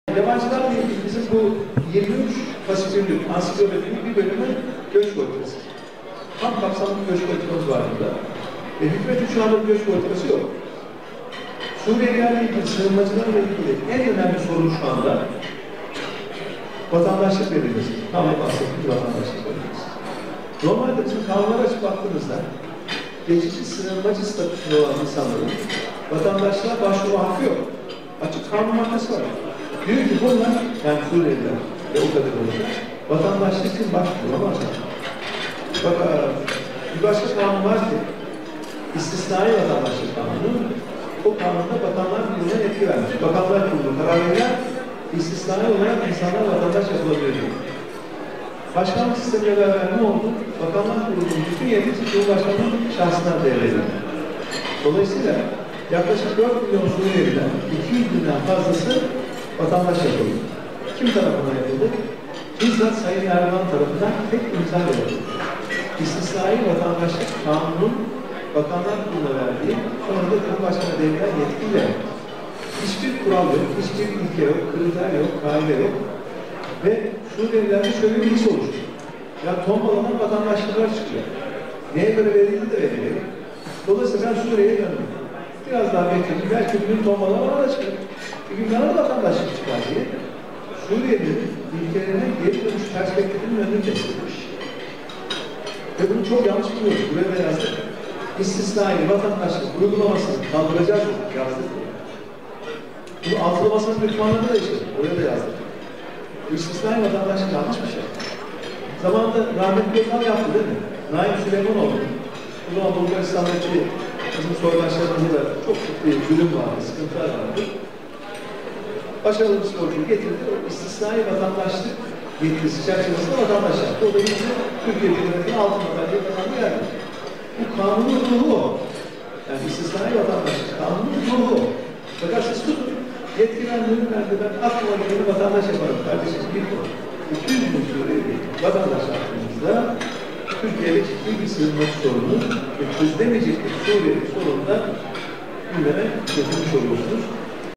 Ali Babacan, “Erdoğan’ın imzasıyla 200 bin Suriyeli’ye vatandaşlık verildi”
200 bin sığınmacıya vatandaşlık verildiğini dile getiren DEVA Partisi Genel Başkanı Ali Babacan, “Yaklaşık 4 milyon Suriyeli ’den 200 bin fazlası vatandaş yapıldı. Bu da bizzat Sayın Erdoğan tarafından tek imza ile yapıldı. 200 bin Suriyeliyi vatandaş yaptığınızda Türkiye’de ciddi bir sığınmacı sorunu gündeme getirmiş oluyorsunuz” dedi.

DEVA Partisi’nin düzenlemiş olduğu iftar programı sonrası gazetecilerin sorularını yanıtlayan DEVA Partisi Genel Başkanı Ali Babacan sığınmacılarla ilgili soruya cevaplayarak, “Sığınmacılarla ilgili en önemli sorun vatandaşlık verilmesi. Bizim kanunları açıp baktığınızda geçici sığınmacı statüsünde olan insanların vatandaşlığa başvuru hakkı yok. Yaklaşık 4 milyon Suriyeli ’den 200 bin fazlası vatandaş yapıldı. Bu da bizzat Sayın Erdoğan tarafından tek imza ile yapıldı. İstisnai vatandaşlık kanunun bakanlar kuruluna verdiği şu anda Cumhurbaşkanına verilen yetkiyle hiçbir kural, ilke ve kriter olmadan verildi. Vatandaşlığın neye göre verildiği de belli değil. Bu da Suriyelilerin ülkelerine geri dönüş perspektifini kesti. Biz istisnai vatandaşlık uygulamasını kaldıracağız. Bu yanlış bir şey. 200 bin Suriyeliyi vatandaş yaptığınızda Türkiye’de ciddi bir sığınmacı sorunu gündeme getirmiş oluyorsunuz” ifadelerini kullandı.